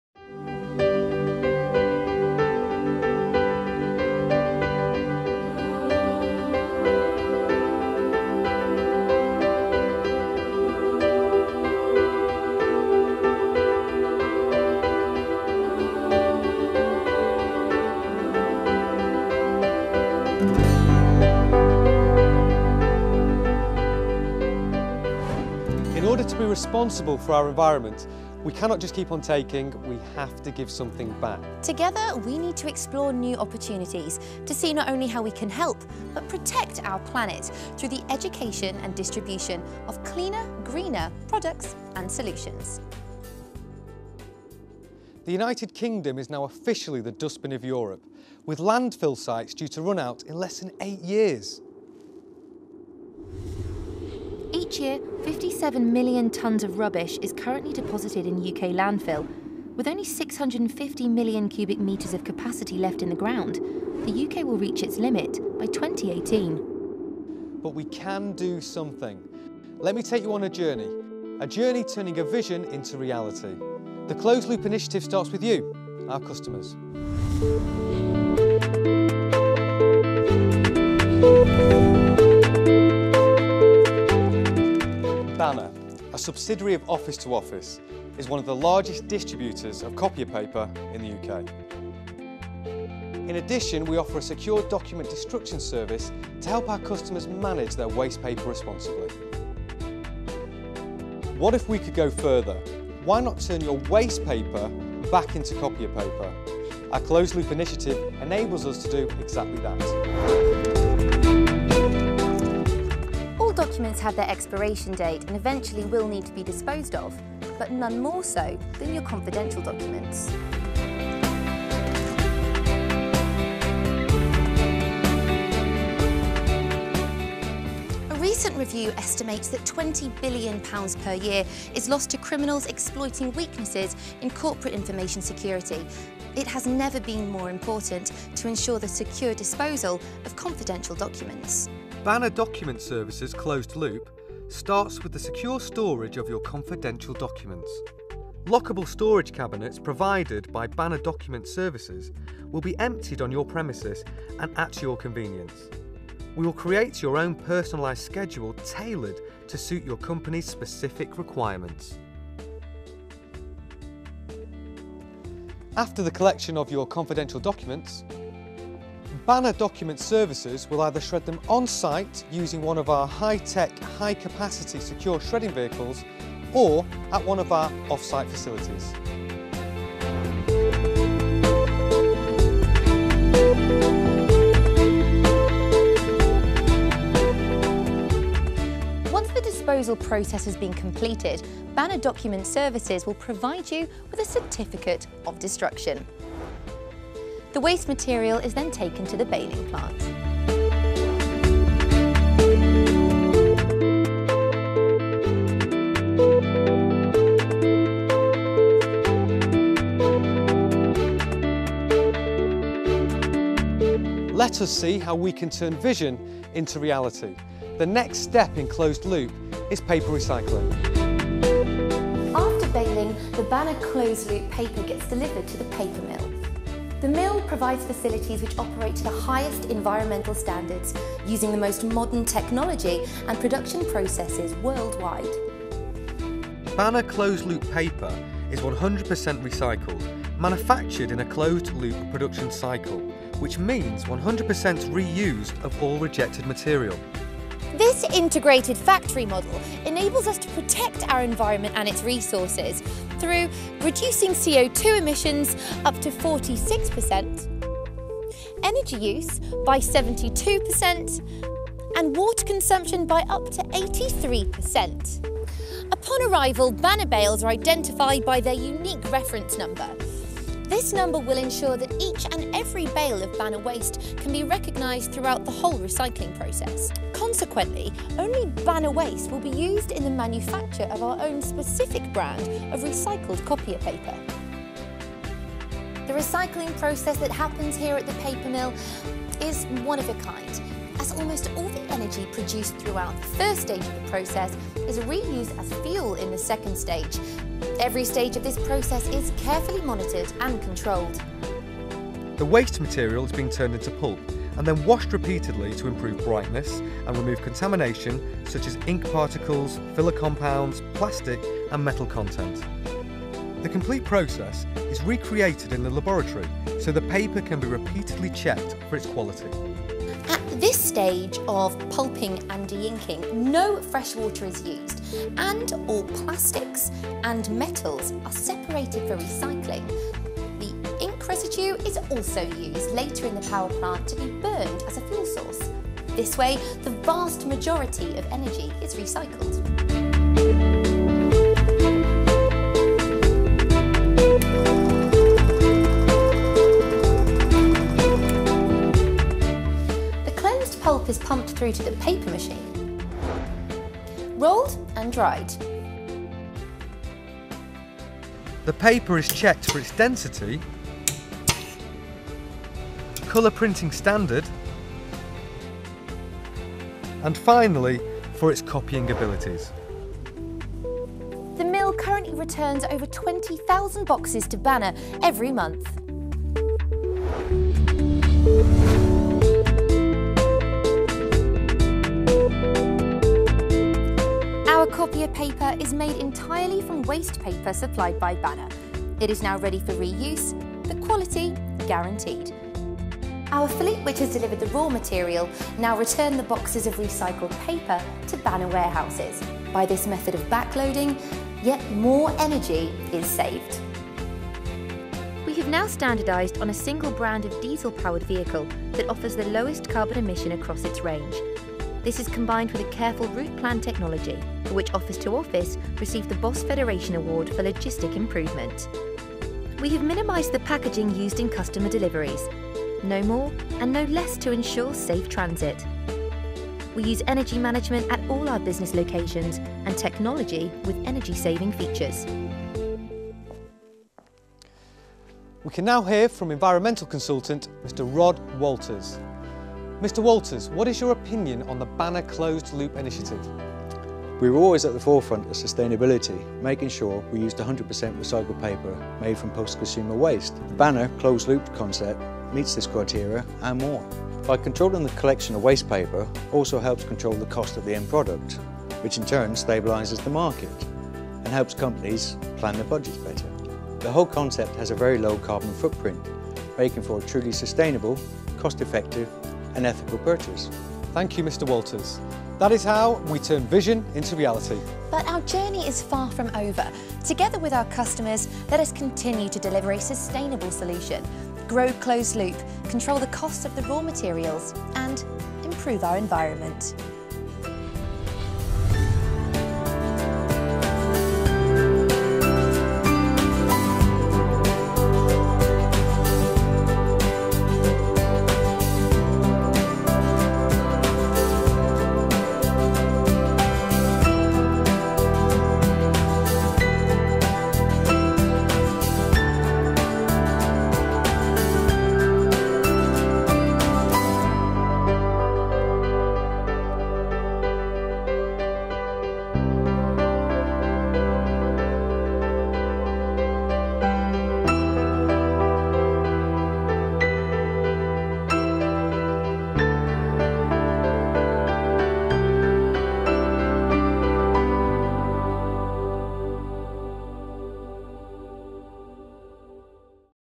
BDS | BANNER DOCUMENT SERVICES UK | Promo film
Promotivni film tvrtke Banner Document Services u svrhu osvještavanja očuvanja okoliša putem pametne i diskretne reciklaže uredske dokumentacije.
Promo video for the Banner Document Services that emphasizes preserving the environment by handling recycling in a smart and discreet way.

Client: BDS
Producer: Mark Morriarty
Direction: Dorotea Vučić
Edit & Post: Kristijan Kaurić
Year: 2010